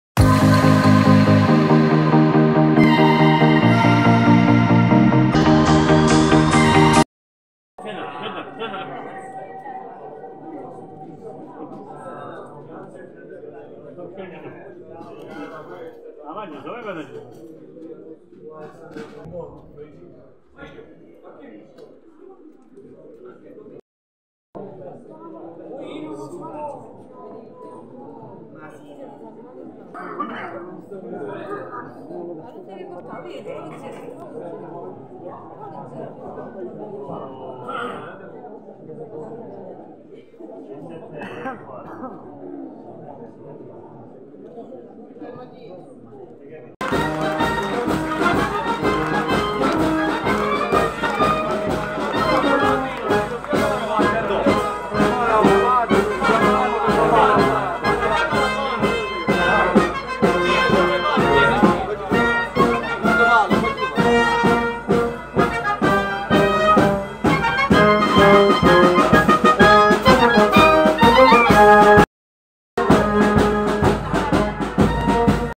Marsicovetere uscita della processione
Uscita della Processione di Sant'Antonio a Marsicovetere (PZ)